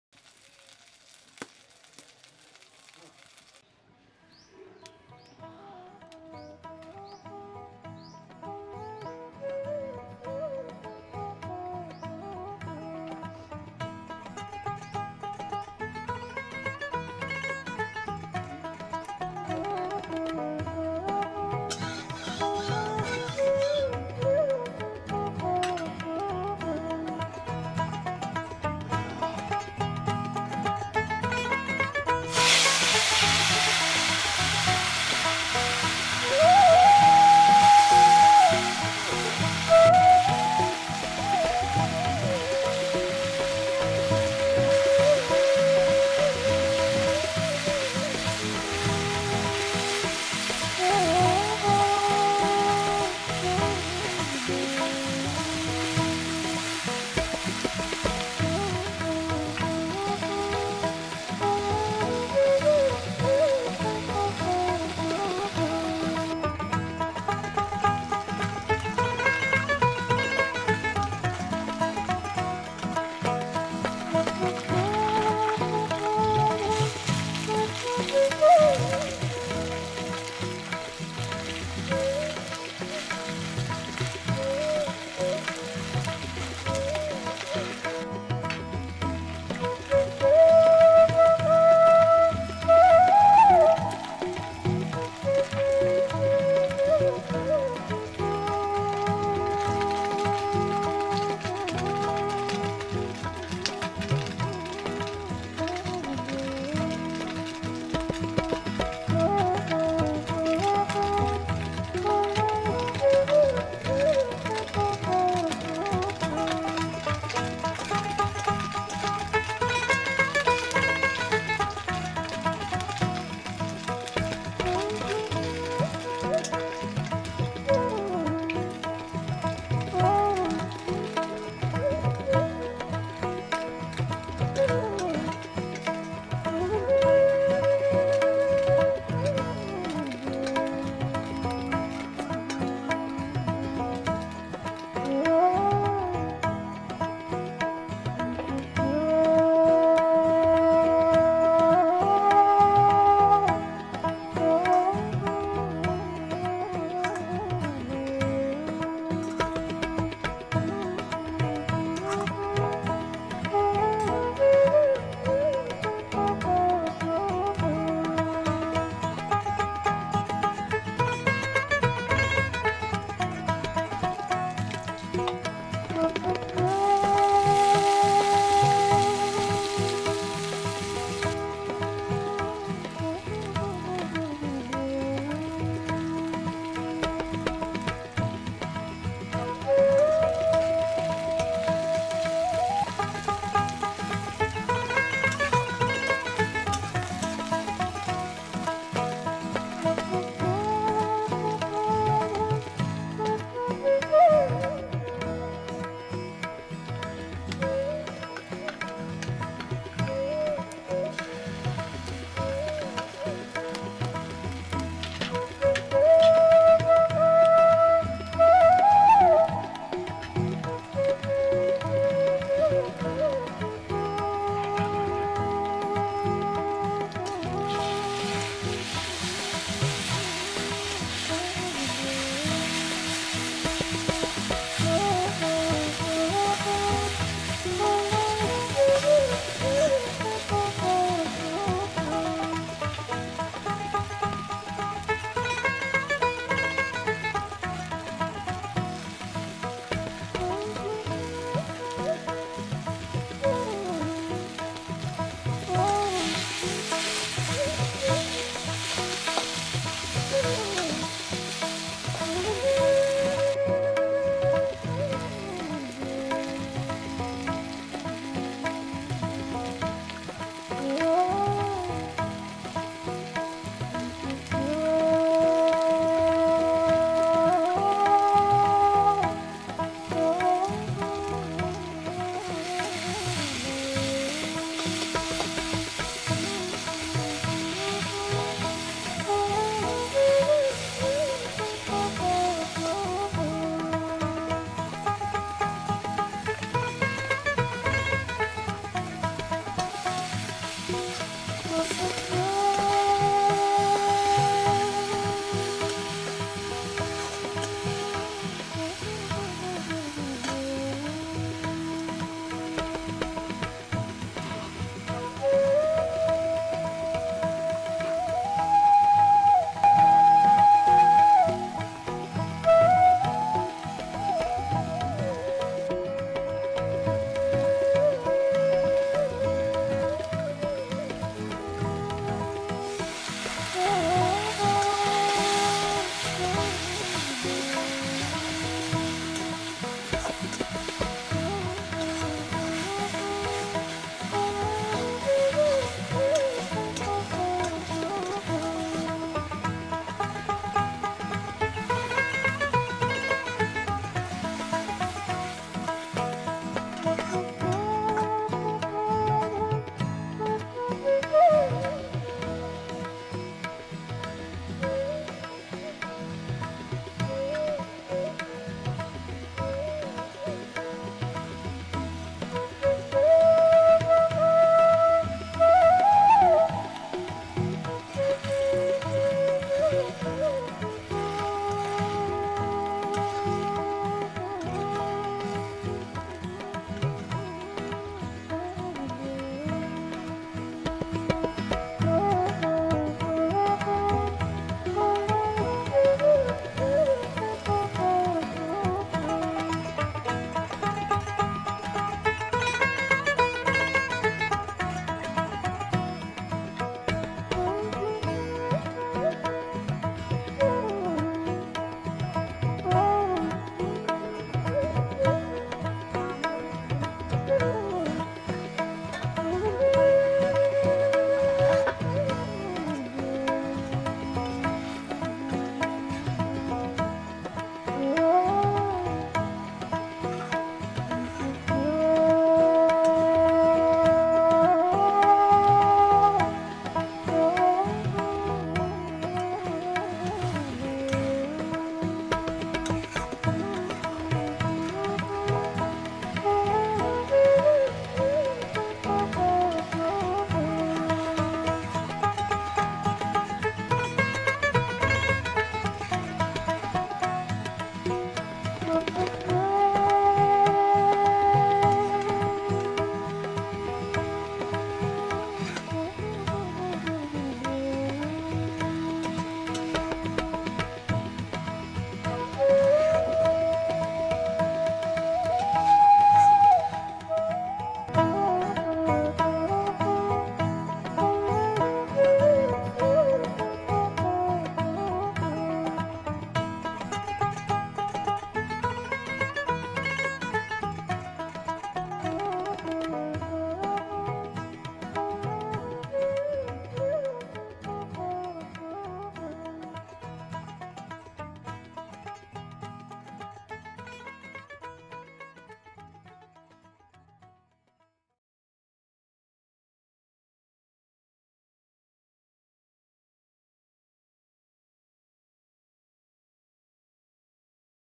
Mourala macher jhal chorchori | মৌরলা/মোলা মাছের ঝাল | Make Mola Carplet Fish village style
Here we have shared the process of making mourola macher recipe / mourola macher jhol. It is basically a type of  bengali fish recipe.
Mourala fish/Mola Carplet Fish is  fresh water fish mostly found  in West Bengal and Orissa Region.

This Recipe is from Orissa

মৌরলা মাছ।

Song: No Copyright Bangla Folk Music Folk Instrumental Music || welcome mood